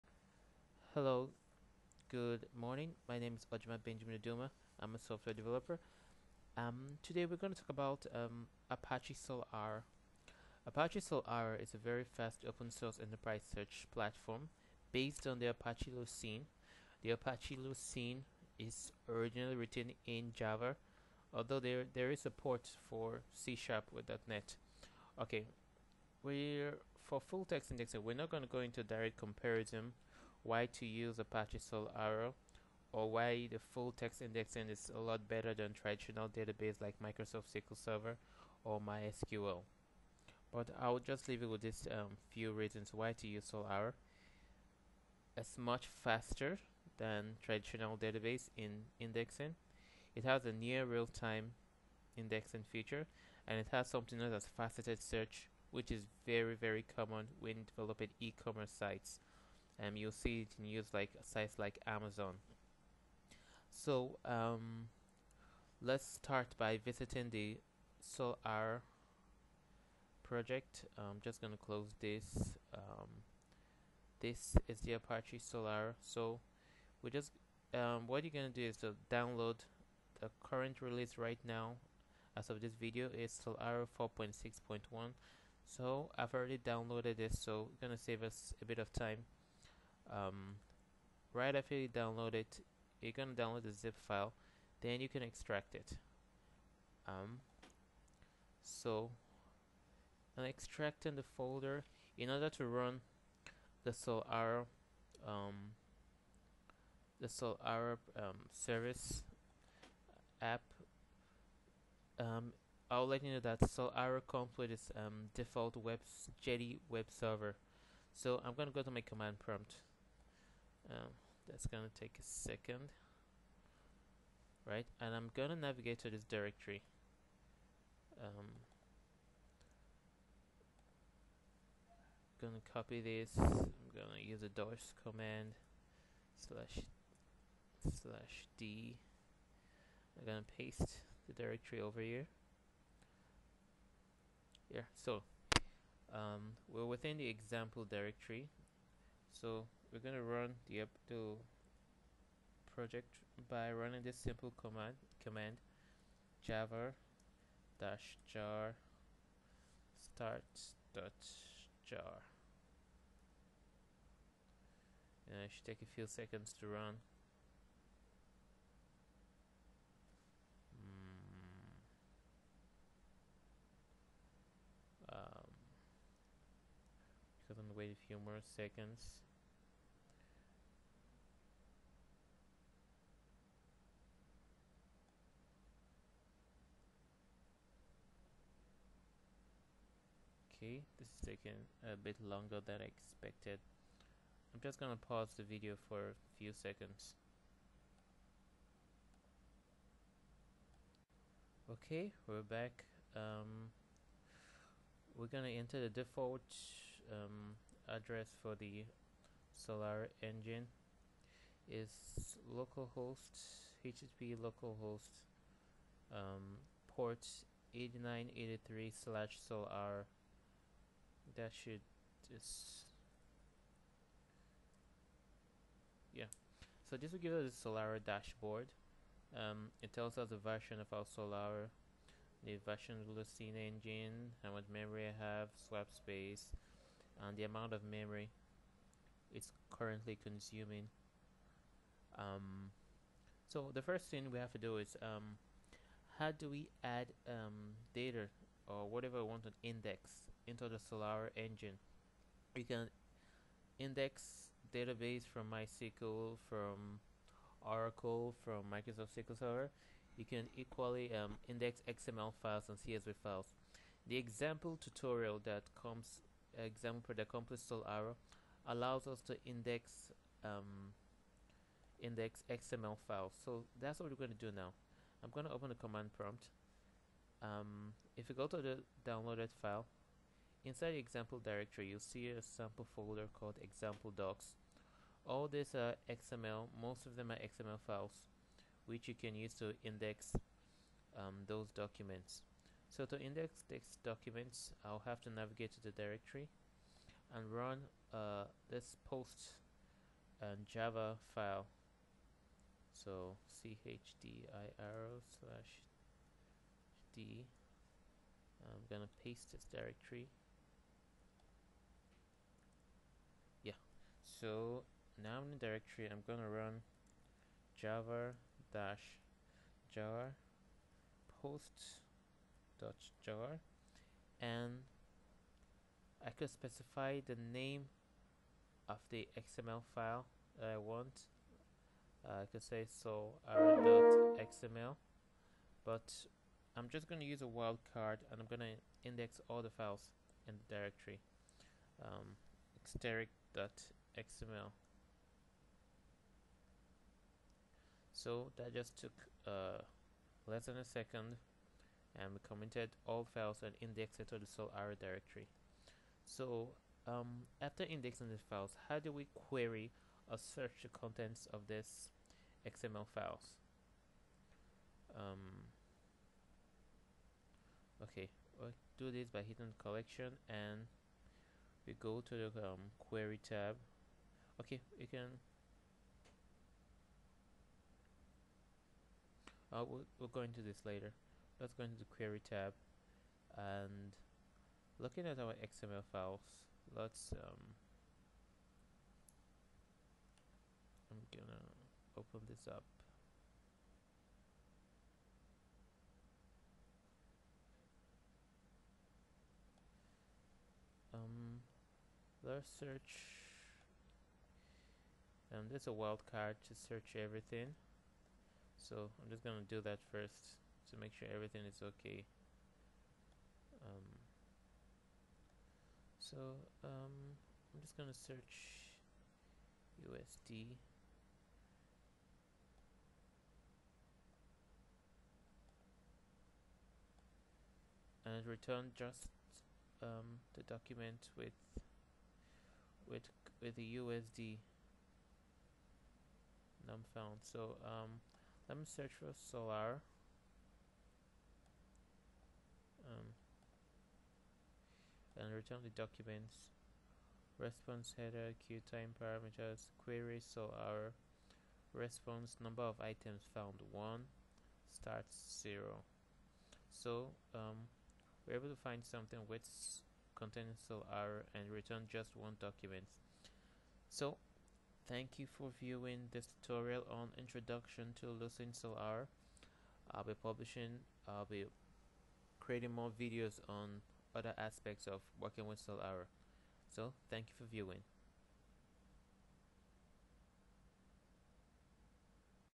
Introduction to Solr
A simple video on how to run solr. A blazing fast open source enterprise search platform